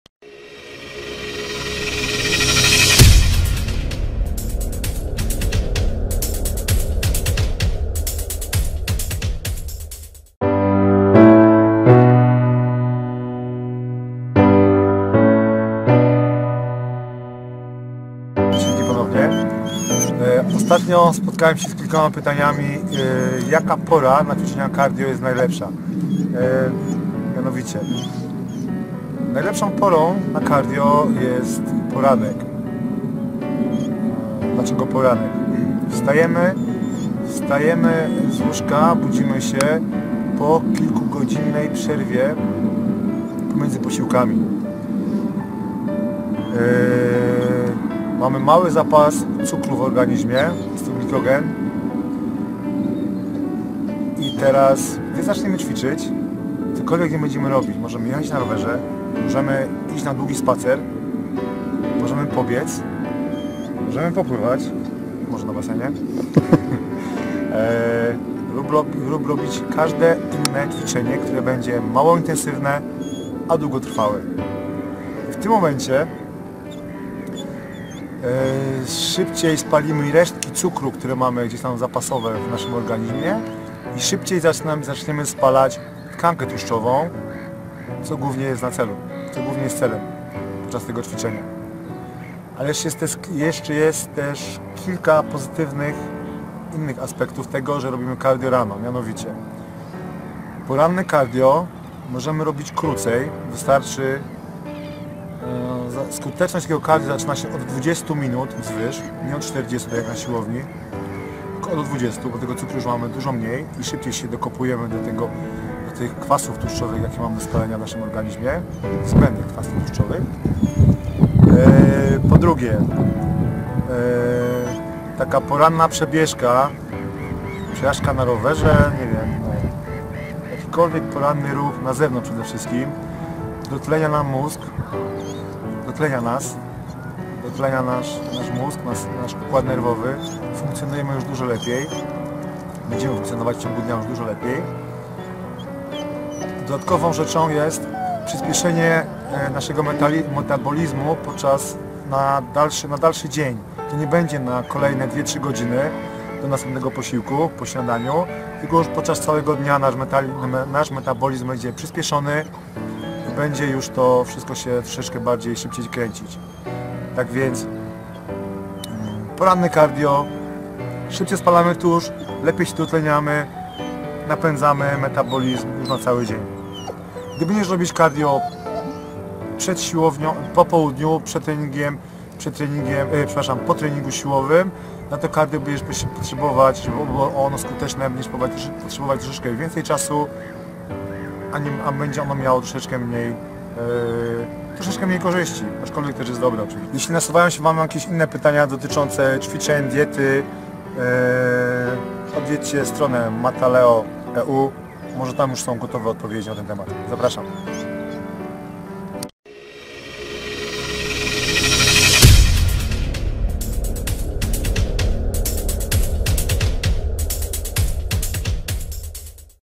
Fitness Cardio
Trening efektywny.
Aby trening spełnił cele, które sobie założyliśmy musimy określić zakres jego intensywności. Znajomość tętna maksymalnego będzie stanowiła podstawę do wyznaczenia dolnej i górnej granicy strefy docelowej, która te cele realizuje.Zapraszamy na stronę Mataleo.